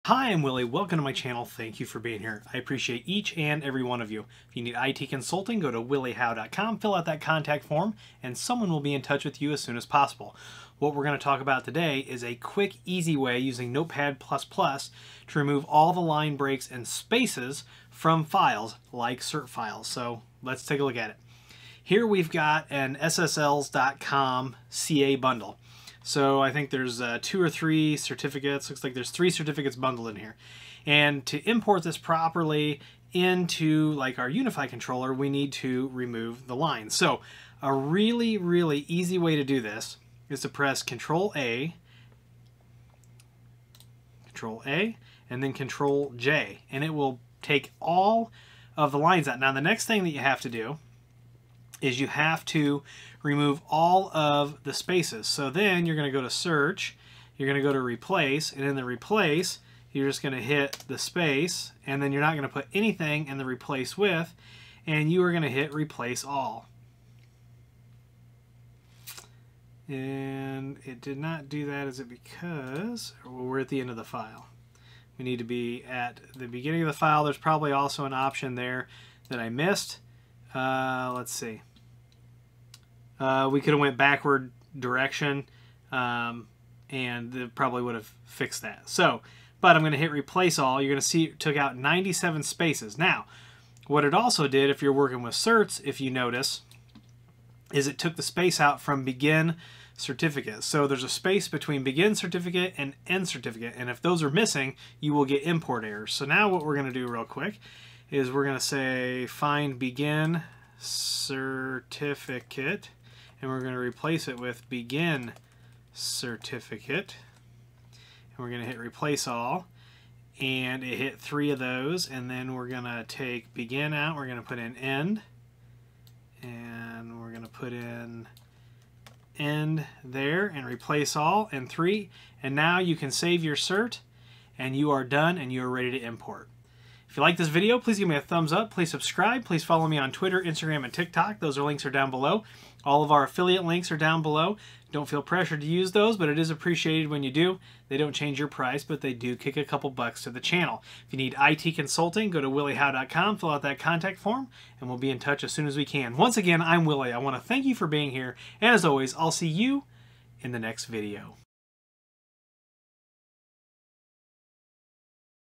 Remove Line Breaks and Spaces From Files Quickly and Easily With Notepad++
Remove line breaks QUICKLY with Notepad++.  Just use ctrl+a and then ctrl+j and all your line breaks are removed!  Then replace all spaces with nothing!

00:00 - Intro
00:10 - Use Notepad++ to remove all the line breaks and spaces in a document QUICKLY and EASILY!
02:16 - Wrap Up